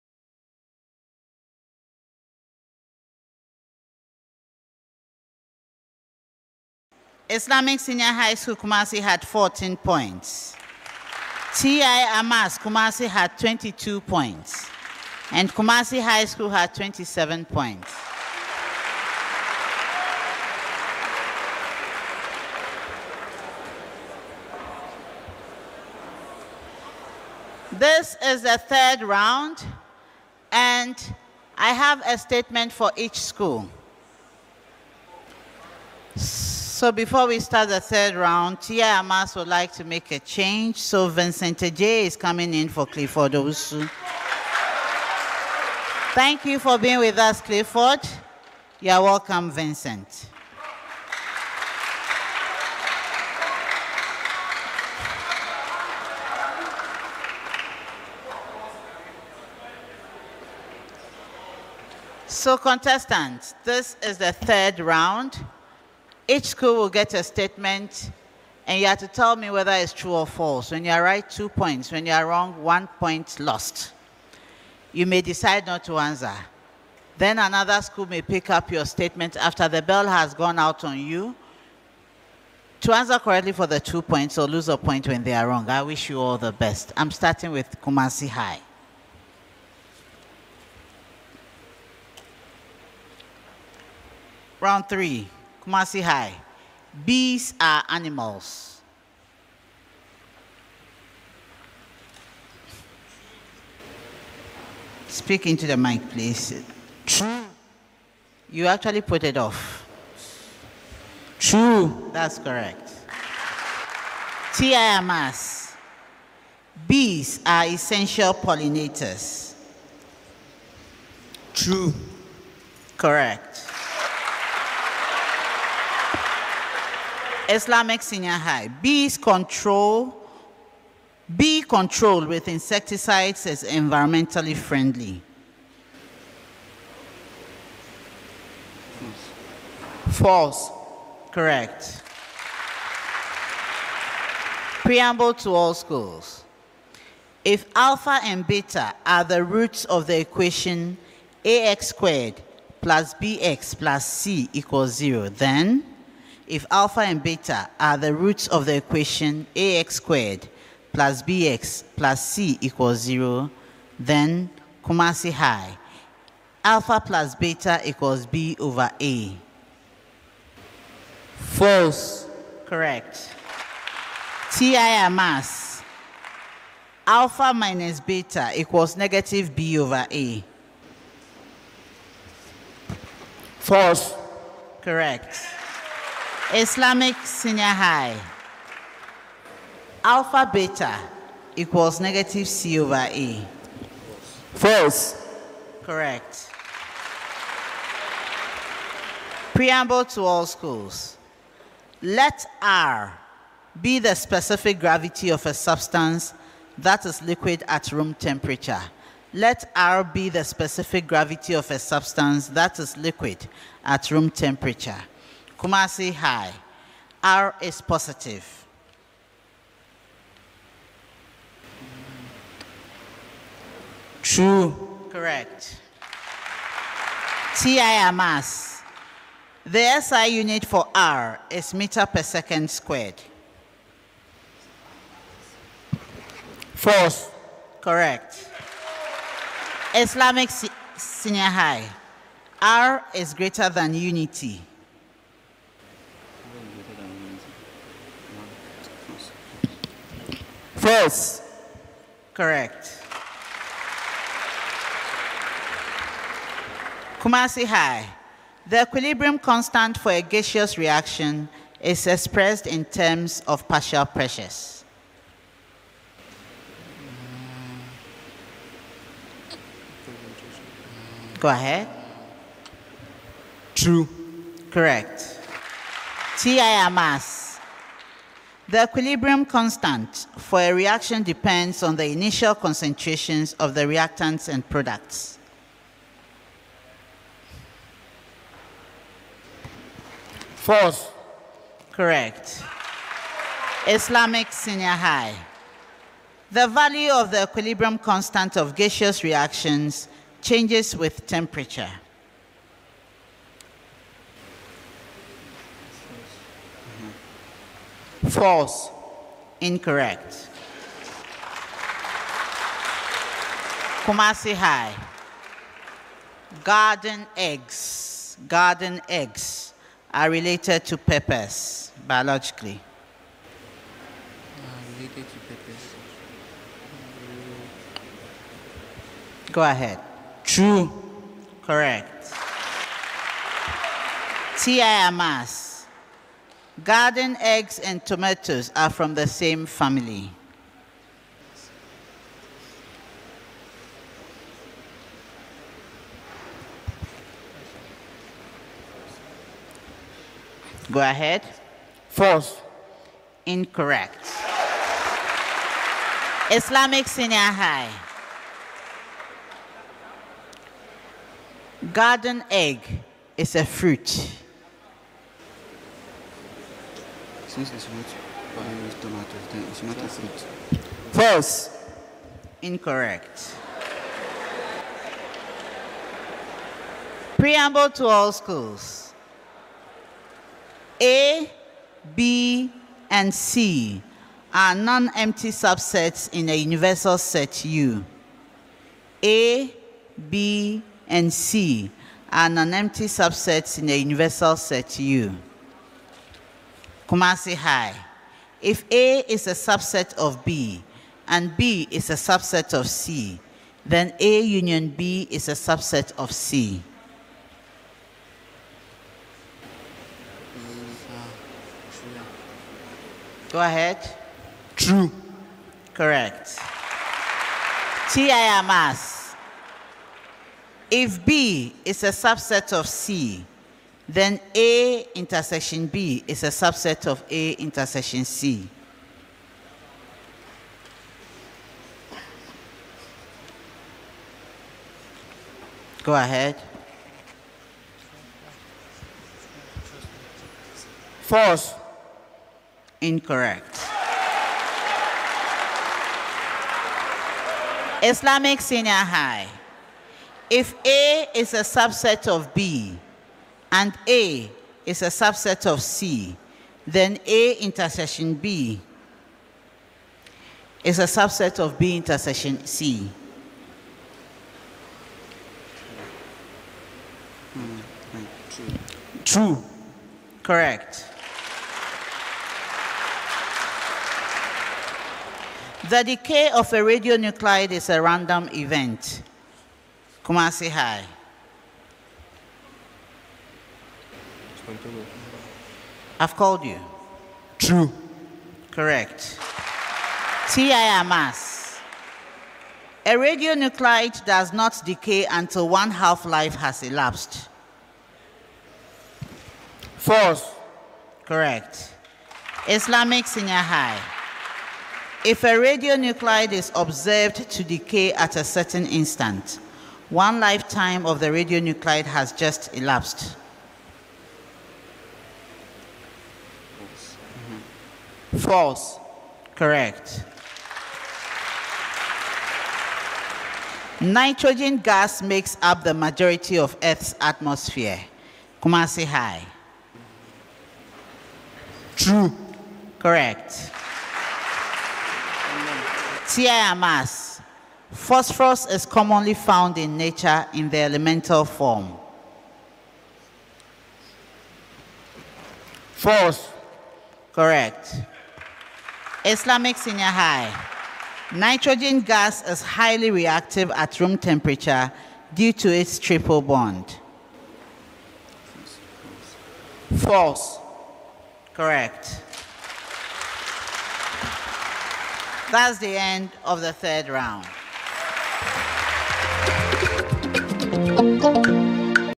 Kumasi High School Dominate True/False in the Ashanti Regional Championship of NSMQ 2024 in Ghana
Hello and welcome to itenert, Kumasi High School dominate True/false to qualify to semi finals of the NSMQ 2024 in Ghana.